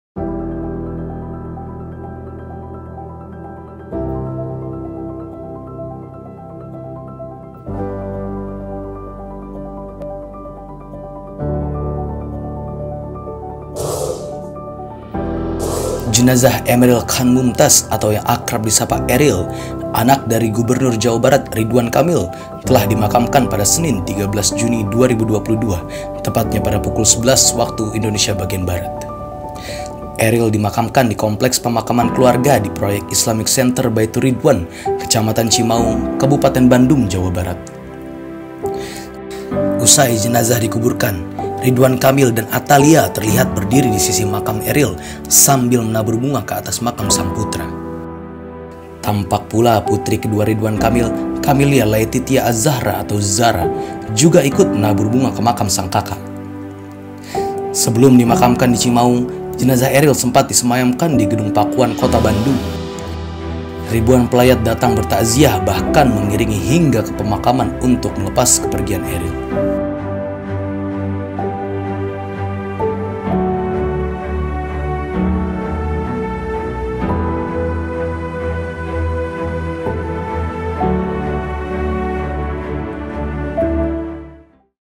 Isak Tangis Mengiringi Pemakaman Emmeril Khan Mumtadz
Jenazah Emmeril Kahn Mumtadz atau yang akrab disapa Eril, anak dari gubernur Jawa Barat Ridwan Kamil telah dimakamkan, pada Senin 13 Juni 2022, pukul 11.00 WIB.

Eril dimakamkan di kompleks pemakaman keluarga, di proyek Islamic Center Baiturridwan, Kecamatan Cimaung, Kabupaten Bandung, Jawa Barat.

Usai jenazah dikuburkan, Ridwan Kamil dan Atalia terlihat berdiri di sisi makam Eril sambil menabur bunga ke atas makam sang putra.

Tampak pula Putri kedua Ridwan Kamil, Camillia Laetitia Azzahra atau Zara, juga ikut menabur bunga ke makam sang kakak.

Sebelum dimakamkan di Cimaung, jenazah Eril sempat disemayamkan di Gedung Pakuan Kota Bandung.

Ribuan pelayat datang bertakziah bahkan ke mengiringi hingga ke pemakaman, untuk melepas kepergian Eril.